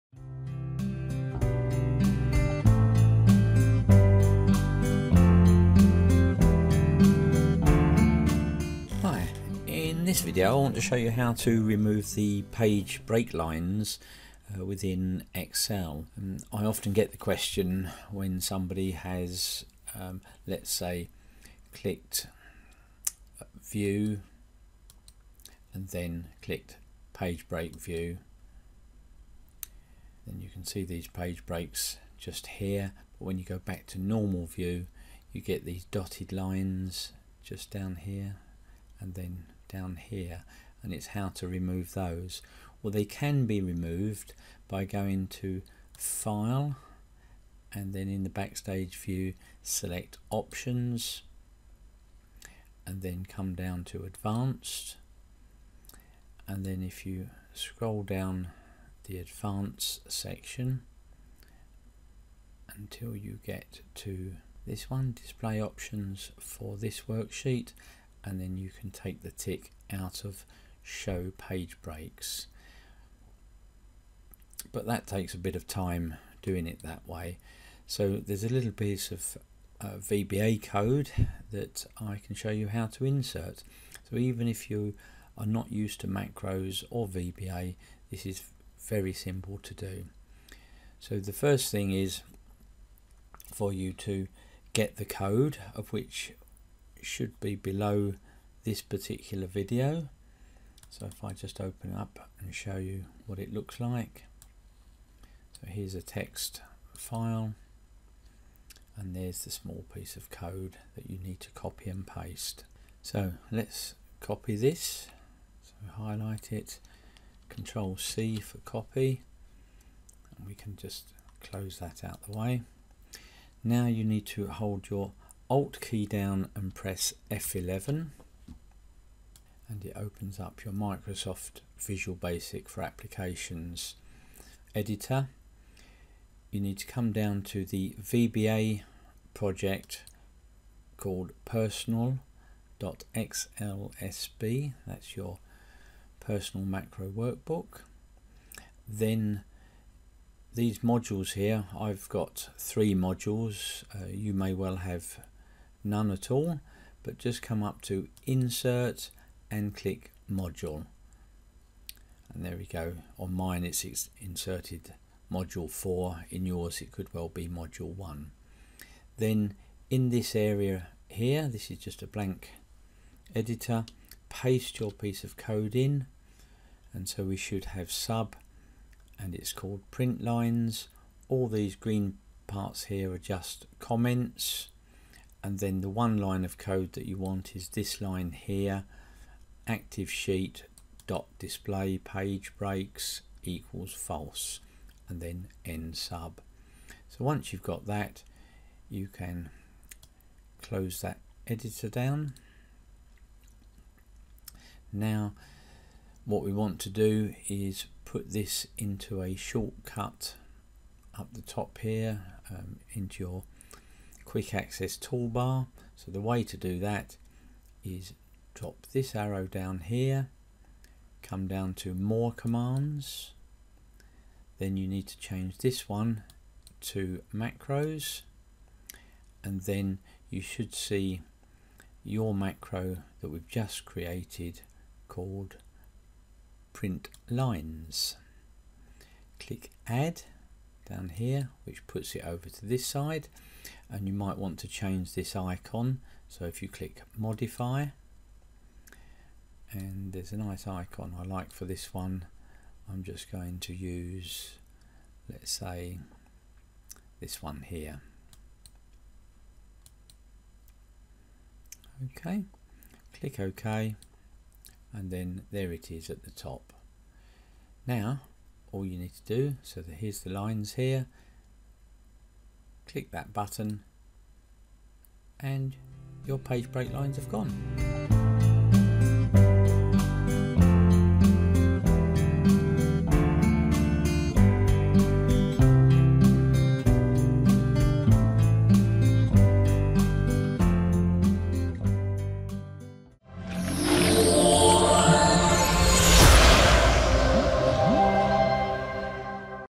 Remove Page Break Lines in Excel
How to remove those annoying dotted page break lines on an Excel Spreadsheet using one line of VBA code. Very easy to do for any level of user.

Sub PrintLines()
'Removes page break dotted lines
'Create in Personal Macro workbook
'PERSONAL.XLSB
'Place a link in Quick Access Tool Bar
'InITLearning.net
ActiveSheet.DisplayPageBreaks = False
End Sub